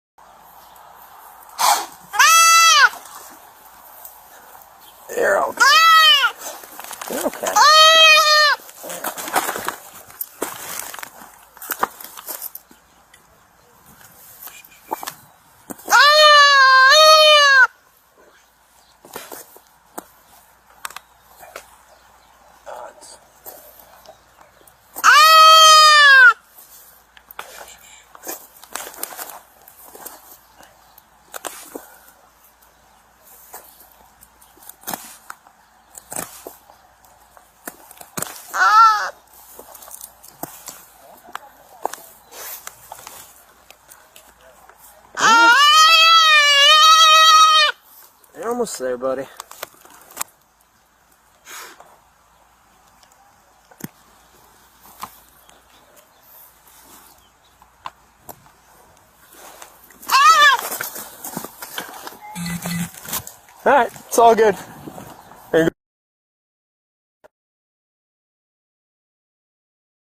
Entangled baby deer freed from backyard soccer net
Video shared by City of Westlake Ohio Police Department shows officers rescuing a baby deer that had become stuck in a backyard soccer net.